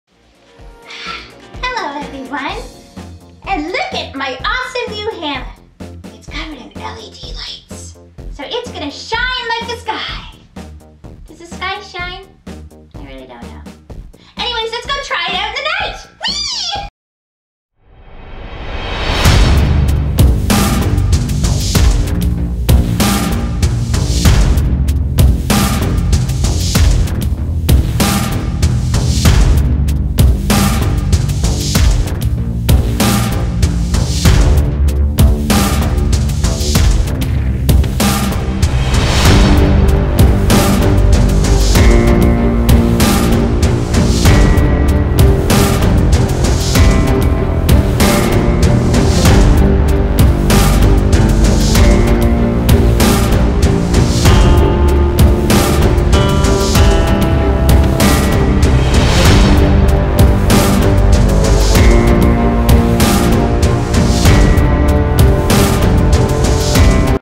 Harley Quinn's Glowing Hammer Spin!
I was a little tipsy as this was filmed late new years eve 2016. Here's my hammer after getting equipped with a light upgrade.